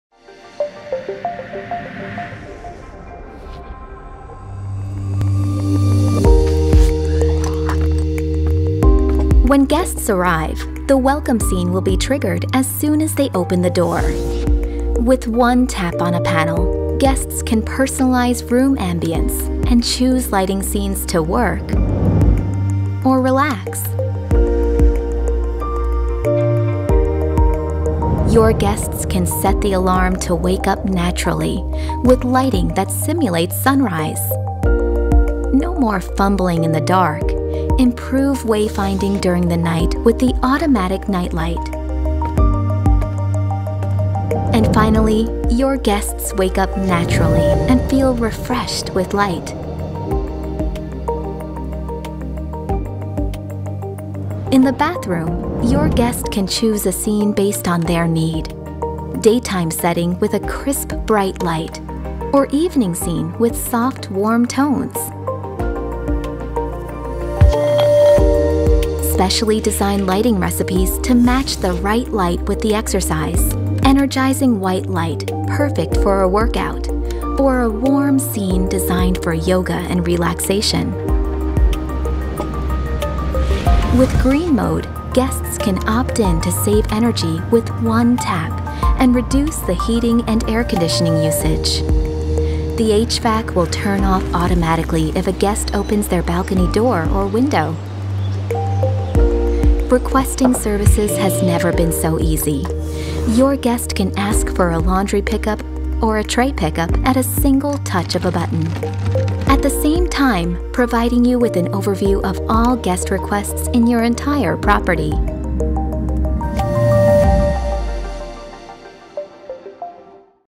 Interact Hospitality. Iluminação LED conectada para os seus hóspedes
Interact Hospitality pode ajudá-lo a melhorar a experiência dos hóspedes no seu hotel. A iluminação bioadaptativa garante conforto durante todo o dia.

Simule um acordar natural pela manhã, forneça energia aos seus convidados na casa de banho ou no ginásio ou reduza os níveis de luz à noite para criar um ambiente ideal para o descanso. O software Interact Hospitality para os quartos de hóspedes integra-se com aquecimento ou ar condicionado, entre outros sistemas. Você pode aceder a todas as informações relevantes através de um único painel de controlo intuitivo que ajudará a simplificar as funções de sua equipa.